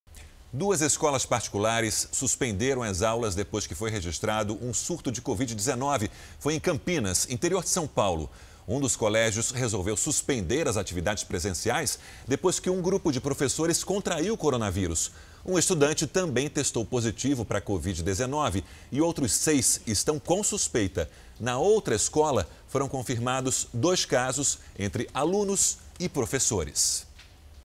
Um dia depois da volta às aulas, escolas do interior de SP registram surto de covid-19
Em Campinas, no interior de São Paulo, duas escolas particulares suspenderam as aulas depois que foi registrado um surto de covid-19. Uma das escolas resolveu suspender as atividades presenciais depois que um grupo de professores contraiu o novo coronavírus. Um estudante também testou positivo para covid-19 e outros seis estão com suspeita. Na outra escola, foram confirmados dois casos, entre alunos e professores. As aulas também foram suspensas.